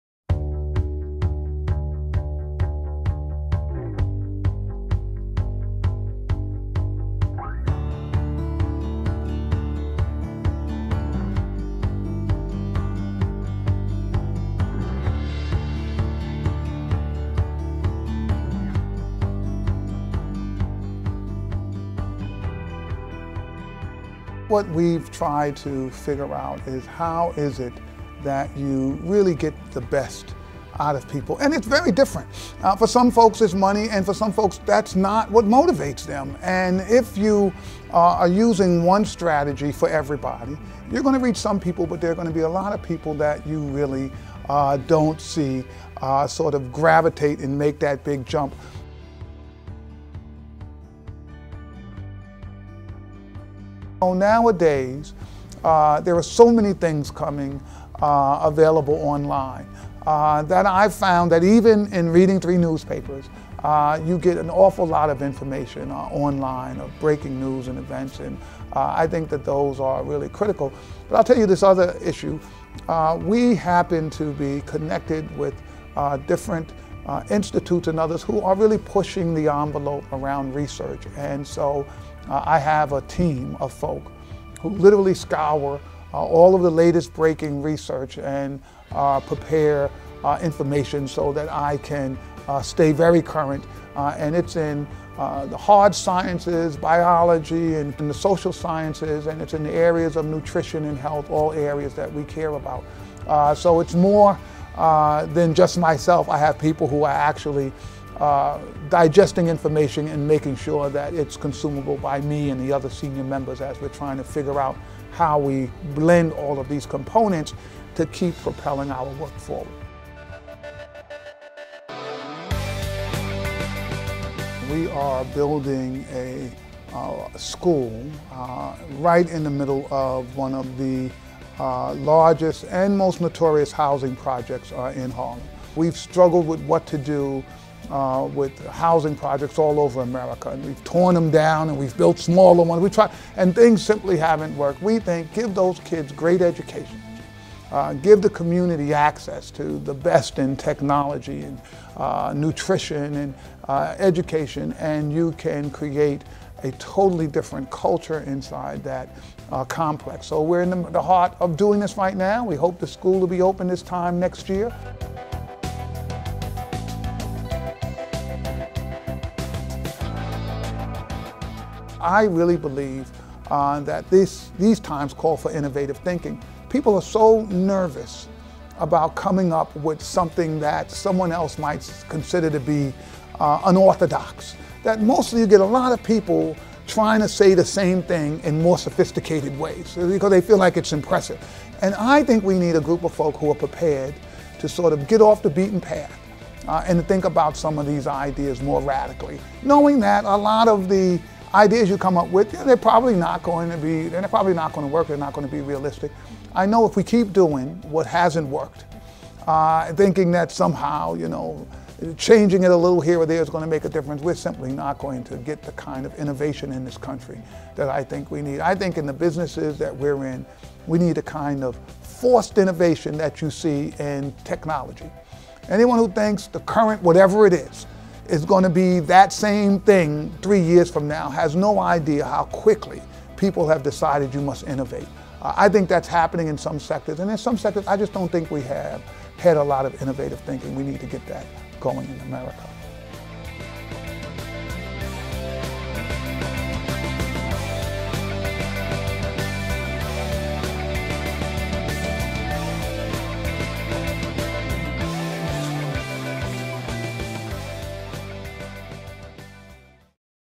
CUP Conversations- Geoffrey Canada
In January, CUP launched a mini-series called "CUP Conversations," where industry trailblazers answer questions on camera about their "lessons on leadership." Take a look at our second highlight video of Geoffrey Canada, Social Activist & Educator and CEO of Harlem Children's Zone.